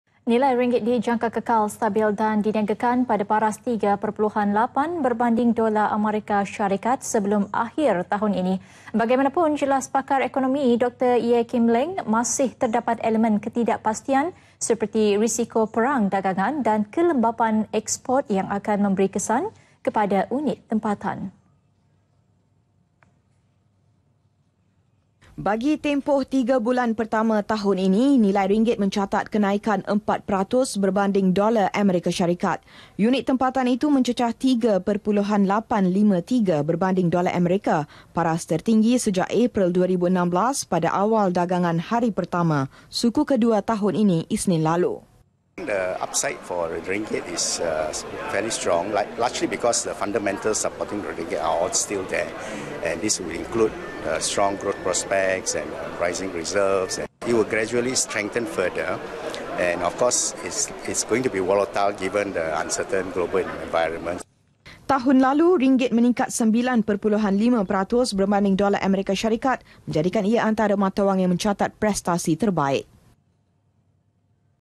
NILAI RINGGIT - DIJANGKA SEKITAR 3.8 BERBANDING DOLAR AMERIKA [05 APRIL 2018]
Nilai ringgit dijangka kekal stabil dan diniagakan pada paras 3.8 berbanding dolar Amerika Syarikat sebelum akhir tahun ini.

Bagaimanapun jelas pakar ekonomi, Dr Yeah Kim Leng, masih terdapat elemen ketidakpastian seperti risiko perang dagangan dan kelembapan eksport yang akan memberi kesan kepada unit tempatan.